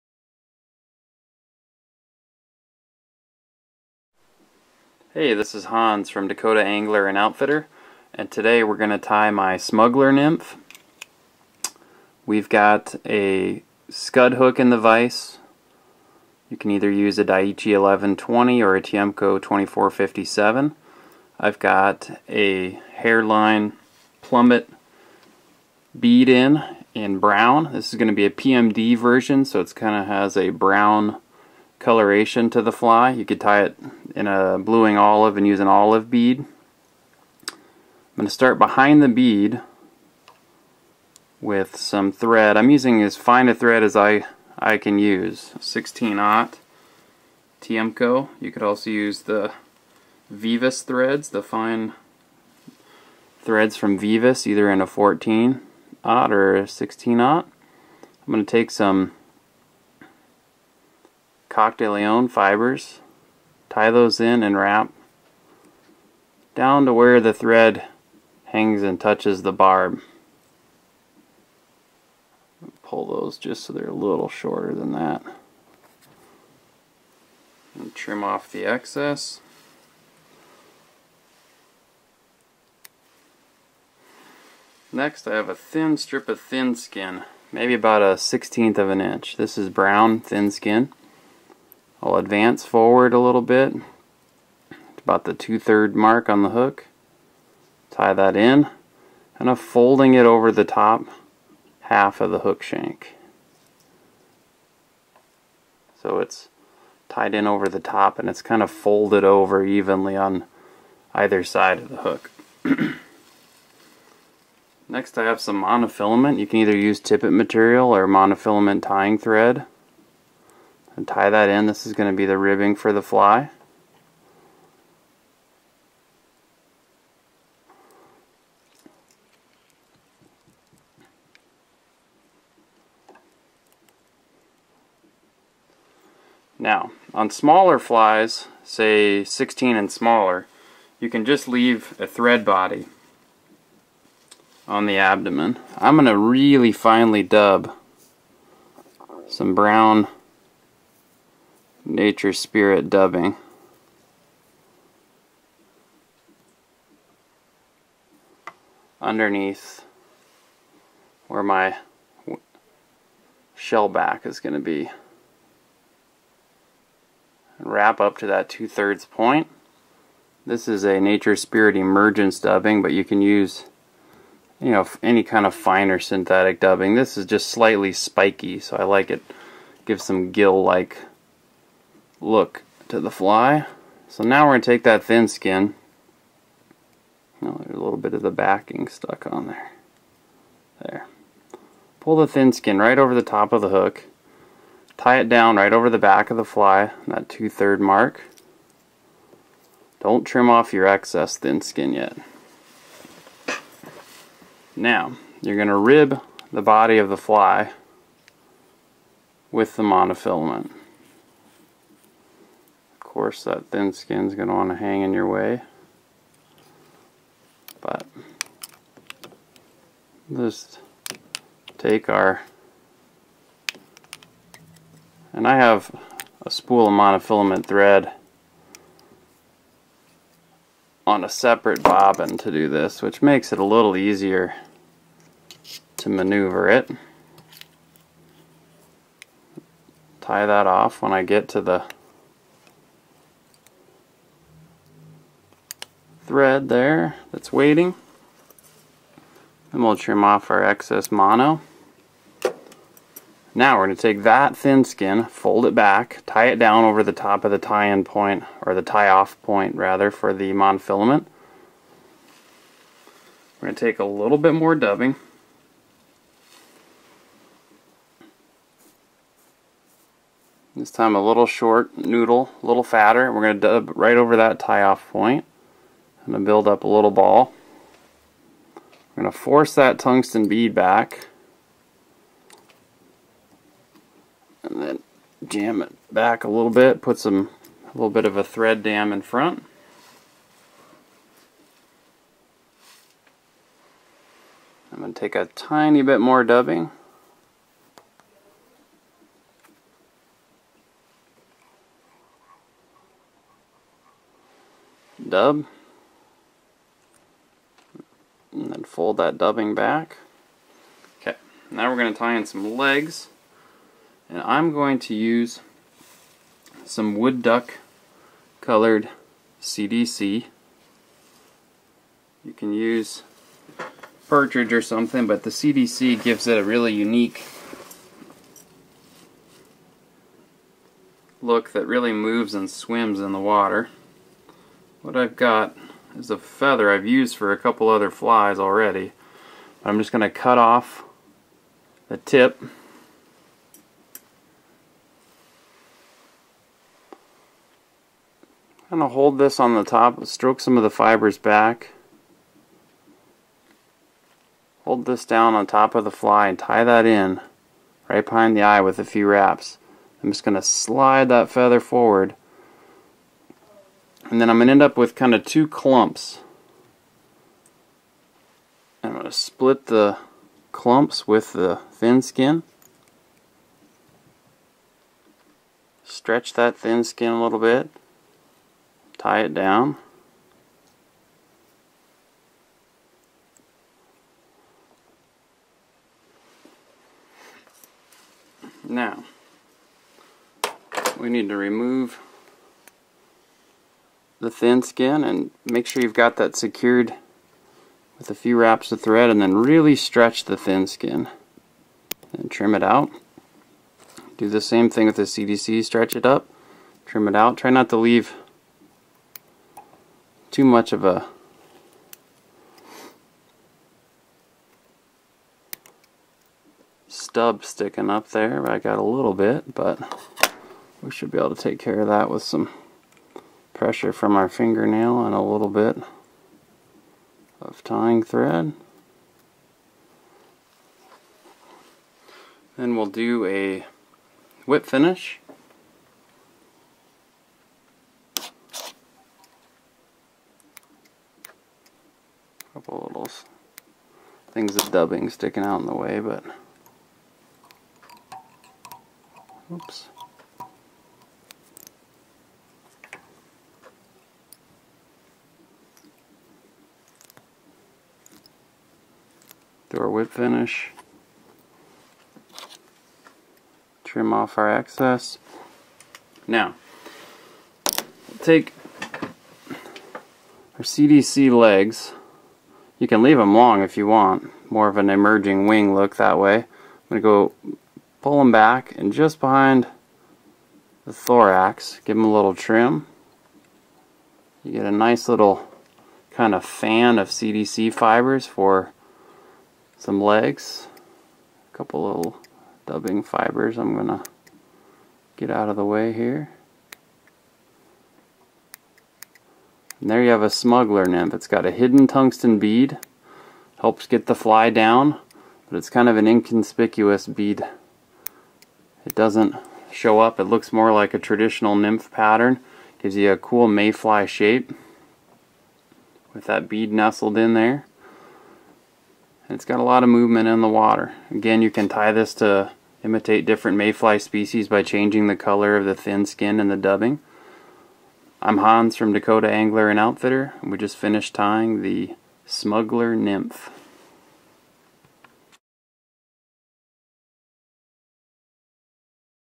Fly Tying with Hans Tungsten Smuggler Nymph
Hans from Dakota Angler ties his Tungsten Smuggler, a great hidden bead pattern that can fool the pickiest fish! Tie it in black, olive, and brown, and give it a shot on your local river, you won't be disappointed.